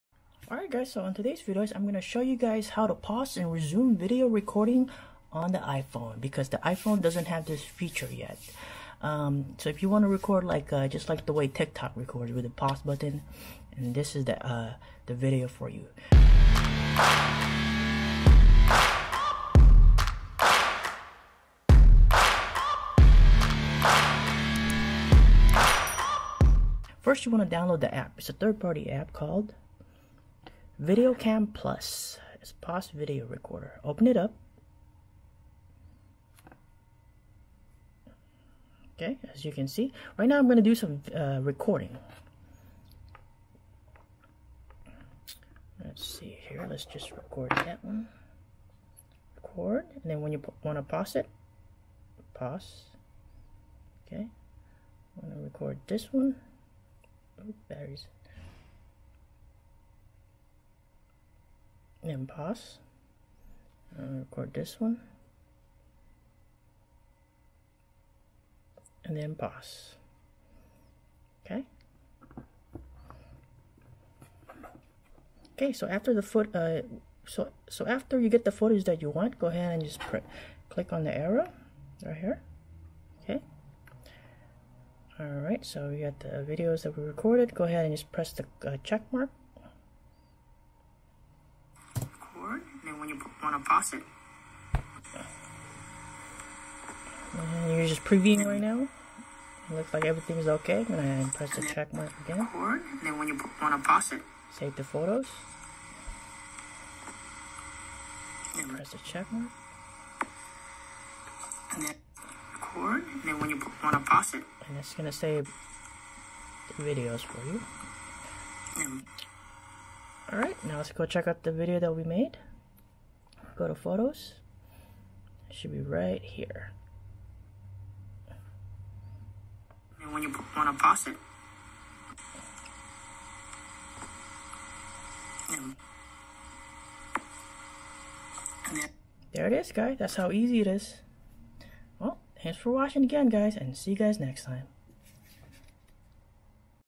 How to Pause and Resume Video Recording on iPhone - iPhone Tips and Tricks
I will show you how to Record Videos With A Pause Button On The iPhone using an app.